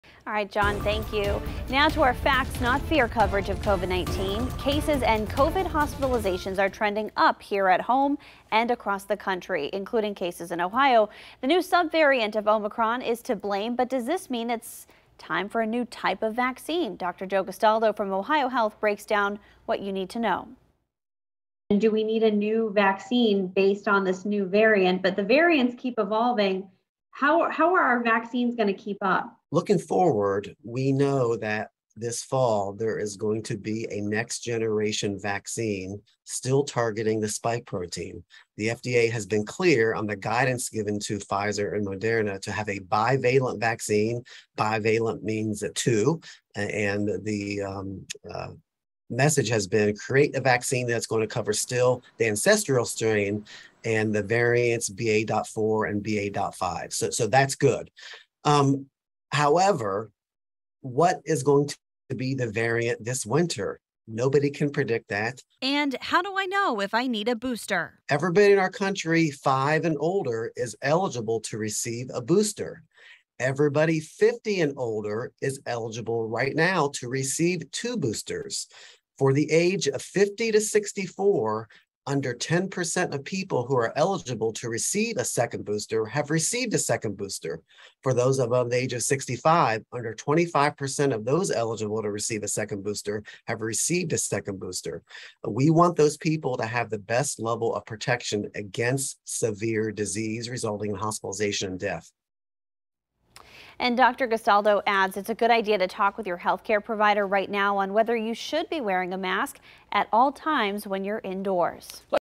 Next generation of COVID-19 vaccines coming this fall
The vaccine will still focus on the spike protein.